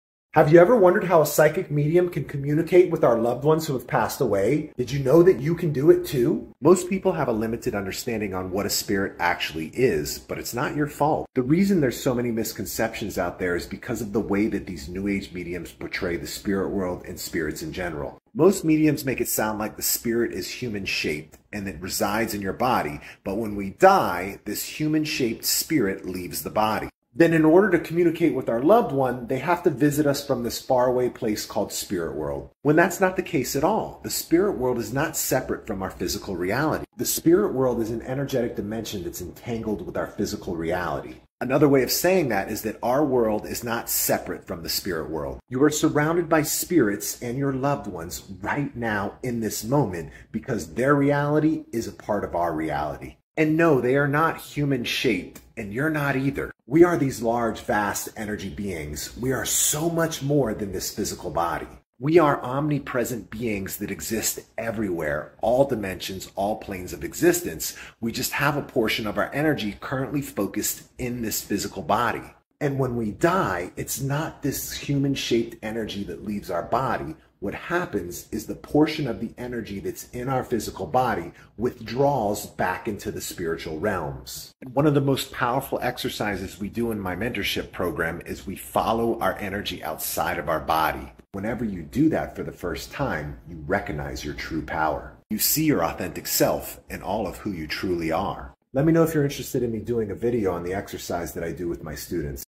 This is How a Psychic-Medium Communicates with Loved Ones Who Passed Away
This is How a Psychic-Medium Communicates with Loved Ones Who Passed Away. A mediumship reading is something anyone can do. Have you ever connected with a loved one or spirit guide?

Follow me to learn how to connect with loved ones who passed away. Let me show you how easy it is!

Everyone has the ability to communicate with spirits, spirit guides and loved ones. You don't have to be a medium or study mediumship. Spirit communication is an innate ability. Everyone is a psychic-medium!